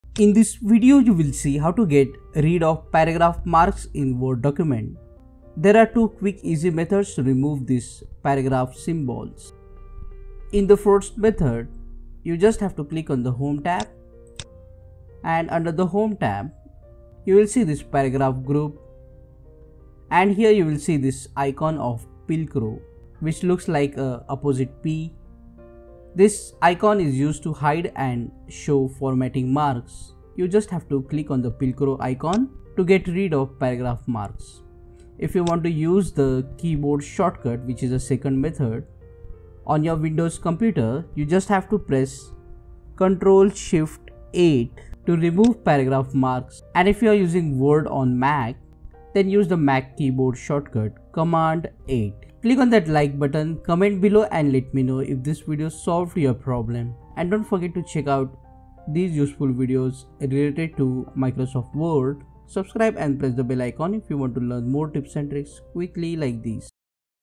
How To Get Rid Of Paragraph Marks In Word
In this video, you will see How to get rid of paragraph marks in Word document. To get rid of paragraph marks also called blue pilcrow icons you can go to Home tab, and click the Show or Hide formatting symbols. you can click this icon to make formatting marks on or off.
You can also use the keyboard shortcut keys Ctrl+Shift+* to turn paragraph marks ON or Off.